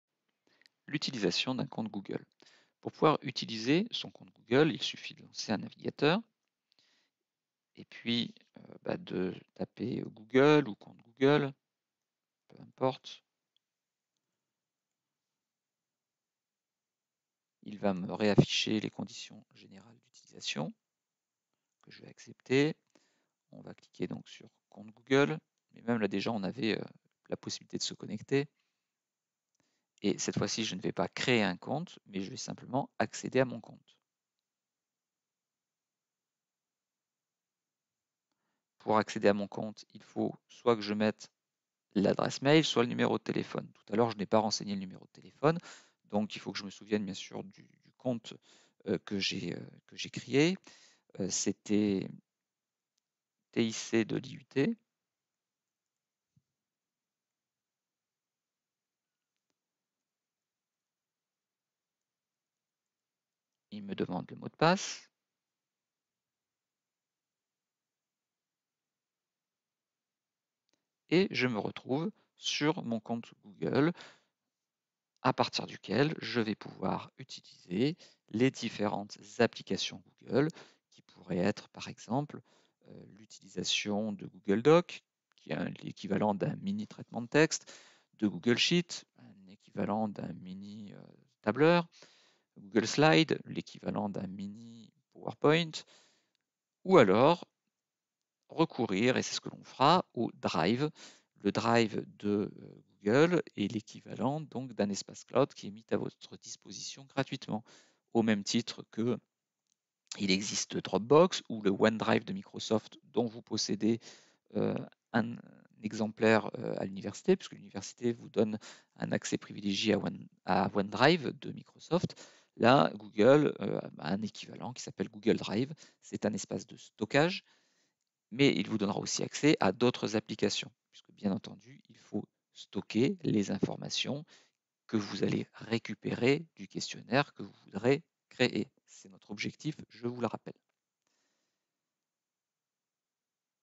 Google Forms 02 - Accéder à mon compte Google
Pour utiliser Google Forms qui est un outil en ligne d'administration de questionnaires, il  faut disposer d'un compte Google.